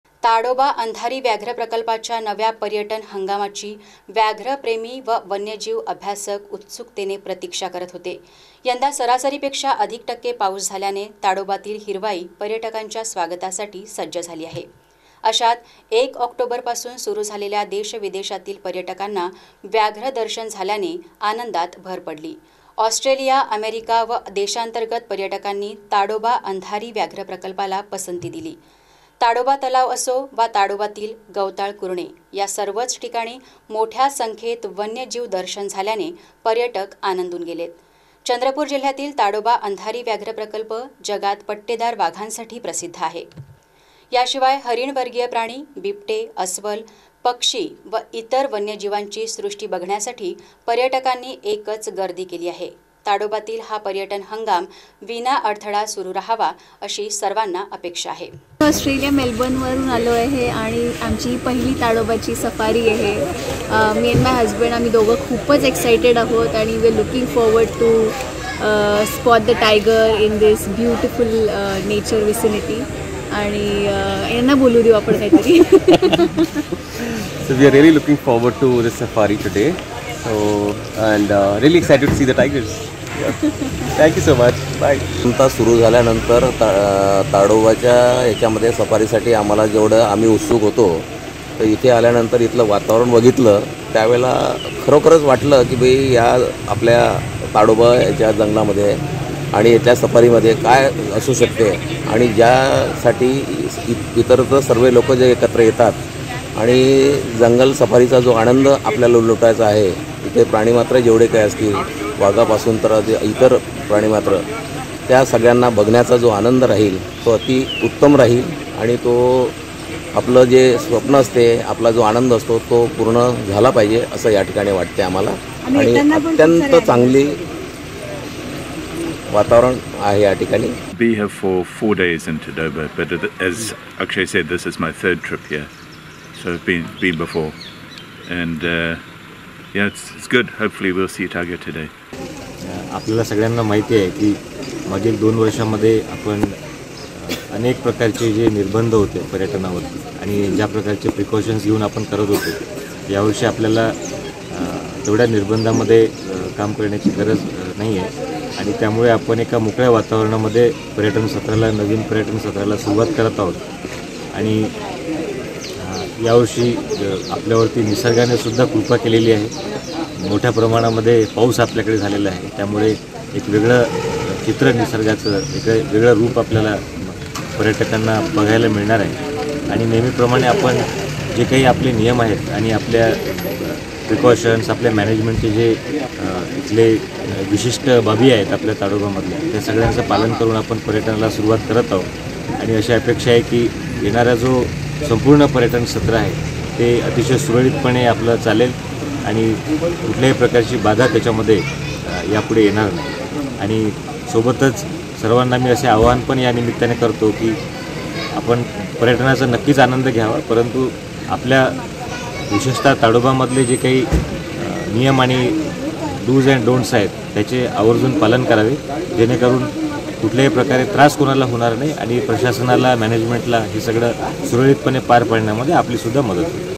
2 October 2022 SearchTv News Chandrapur ताडोबा अंधारी व्याघ्र प्रकल्पात नव्या पर्यटन प्रारंभ
जगप्रसिद्ध ताडोबा अंधारी व्याघ्र प्रकल्पात नव्या पर्यटन हंगामाचा प्रारंभ , देशी-विदेशी पर्यटकांना होताय व्याघ्र दर्शन